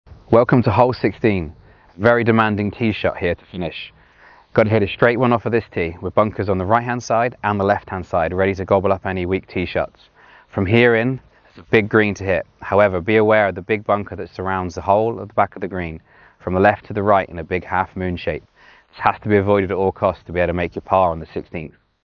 Hole 16 Pro Tip @mountjuliet
"Exploring the Beauty of Mount Juliet Golf Course 🏌️‍♂️ | A Golfer's Paradise"

Description:
Welcome to our virtual tour of Mount Juliet Golf Course, a hidden gem nestled in the heart of Ireland's stunning countryside! 🍀⛳️ In this video, we'll take you on a journey through lush fairways, scenic landscapes, and world-class golfing experiences that make Mount Juliet a must-visit destination for any golfer or nature enthusiast. Join us as we explore the rich history, championship layouts, and unmatched hospitality that define this golfing paradise.

Whether you're a seasoned golfer looking for a challenging round or simply seeking a serene escape in nature, Mount Juliet Golf Course has something special for everyone. Discover the magic of this award-winning golf course, designed by the legendary Jack Nicklaus, and soak in the breathtaking views of the Nore River Valley.

🏞️ Key Highlights:
✅ Championship Layouts by Jack Nicklaus
✅ Pristine Fairways and Greens
✅ Stunning Views of the Nore River Valley
✅ Rich History and Heritage
✅ Impeccable Hospitality

Experience golfing like never before at Mount Juliet, where the love for the game meets the beauty of nature. Whether you're planning your next golf trip or just want to escape into the tranquility of this Irish gem, this video is your virtual ticket.